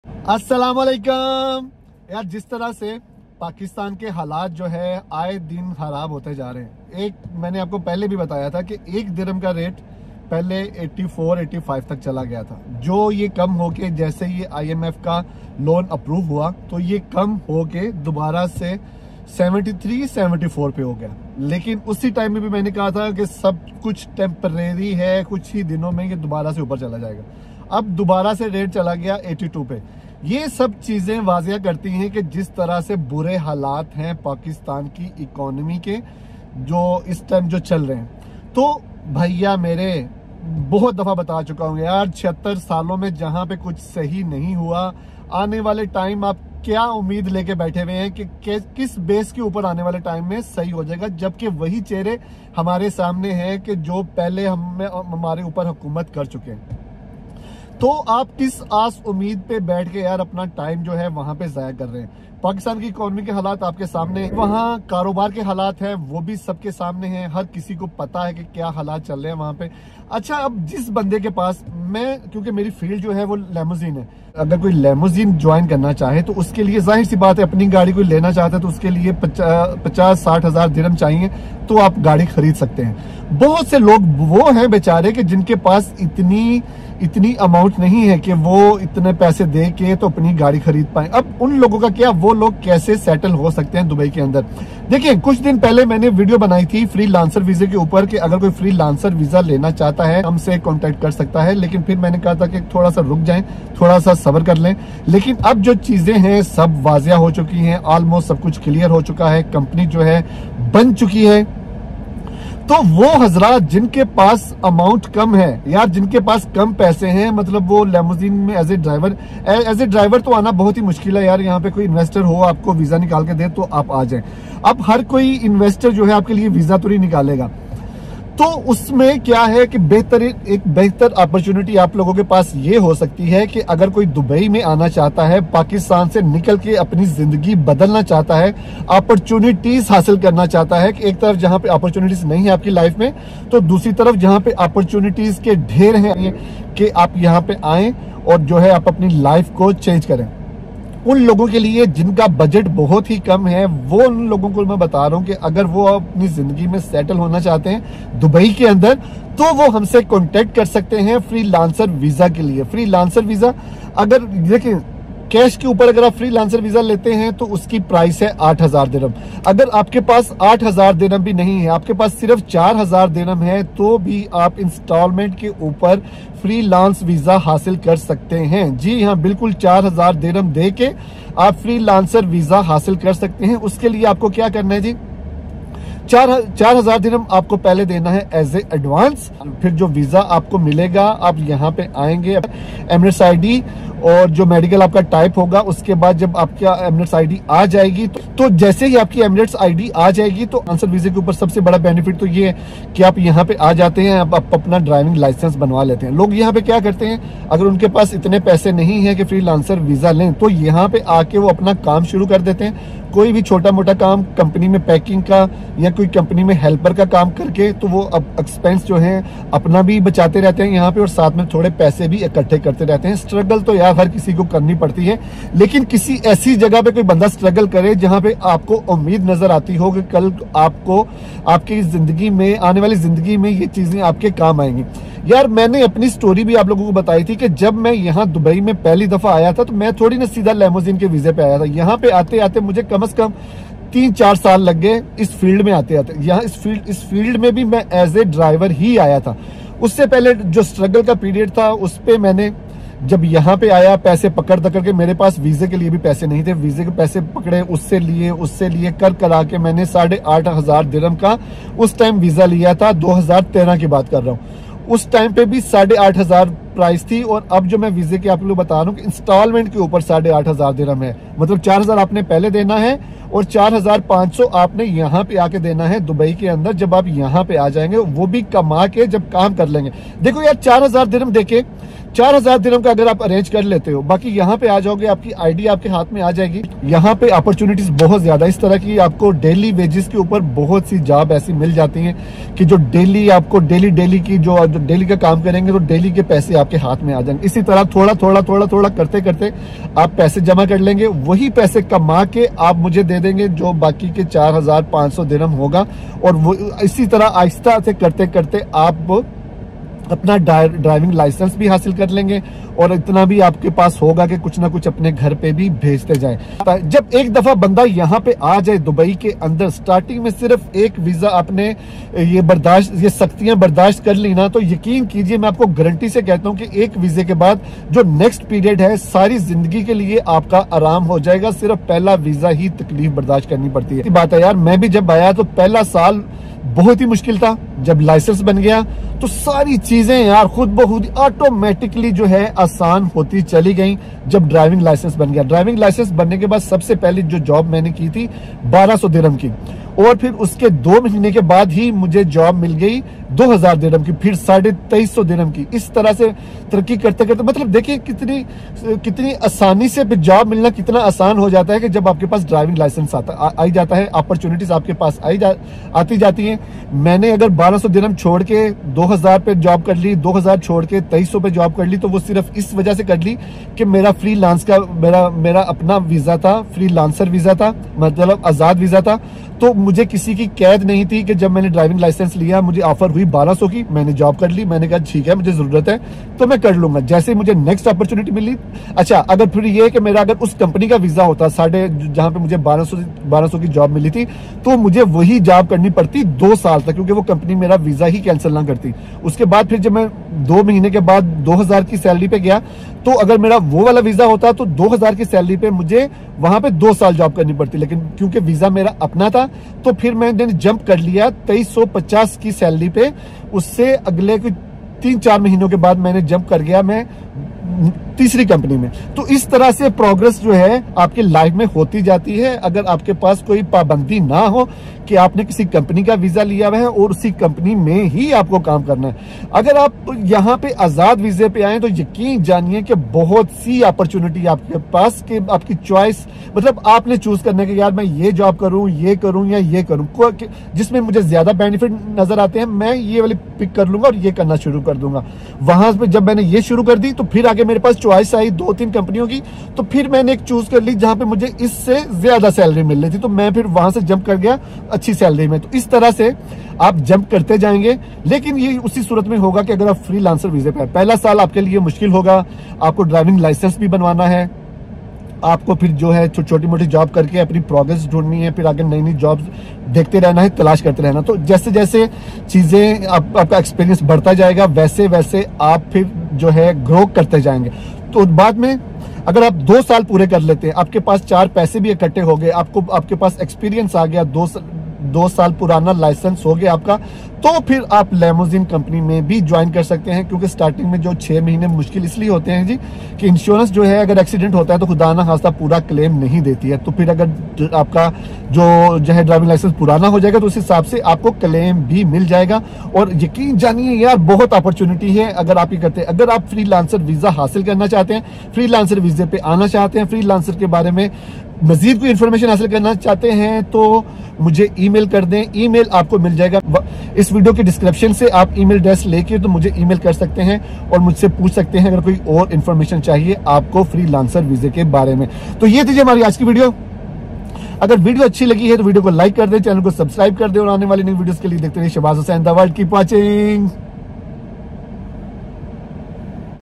Dubai Freelancer Visa | Easy Cash & Installment Options | Your Gateway to Working in Paradise!
Are you an aspiring freelancer looking to work in the vibrant city of Dubai? Look no further! In this video, we unveil the exciting opportunities of obtaining a Freelancer Visa in Dubai, with the added convenience of cash and installment payment options.

Dubai, known for its stunning skyscrapers, luxurious lifestyle, and bustling economy, has emerged as a hub for freelancers from around the world. Whether you're a talented designer, skilled programmer, creative writer, or any other professional seeking to establish yourself in a thriving market, the Dubai Freelancer Visa opens up a world of possibilities for you.

Worried about upfront costs? Fear not! We'll guide you through the process of securing your Freelancer Visa using flexible cash and installment payment plans, making it easier than ever to kickstart your freelancing journey in this dynamic city.

Our team of experts will provide you with valuable insights, step-by-step procedures, and essential tips to ensure a smooth and hassle-free application process. Say goodbye to uncertainty and confusion, and say hello to your dream opportunity in Dubai!

Don't miss out on this chance to live and work in a city that perfectly blends culture, innovation, and success.

Let's unlock the doors to success together in Dubai!